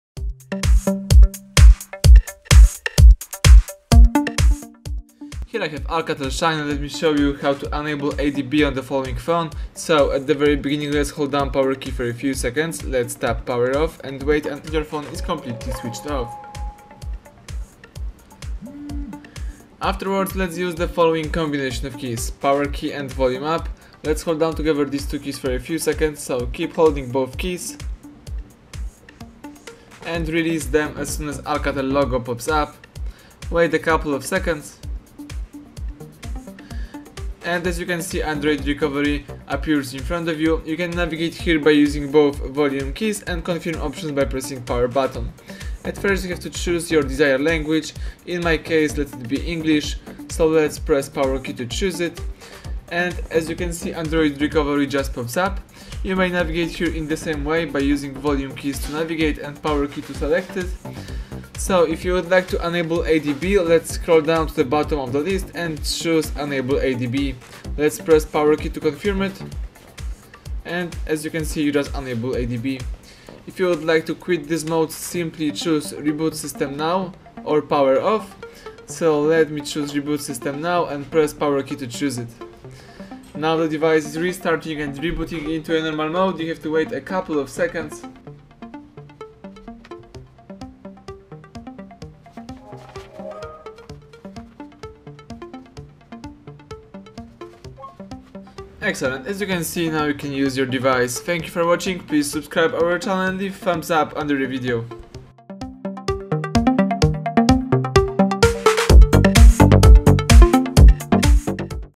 How to Enable ADB in ALCATEL Shine Lite - Use ADB
How to enable ADB in ALCATEL Shine Lite? How to activate ADB from Recovery Mode? How to use ADB in ALCATEL Shine Lite?

ADB (Android Debug Bridge) is a command-line utility. By using ADB you can control your smartphone over USB from a PC, copy files back and forth, install and uninstall apps, run shell commands, and more.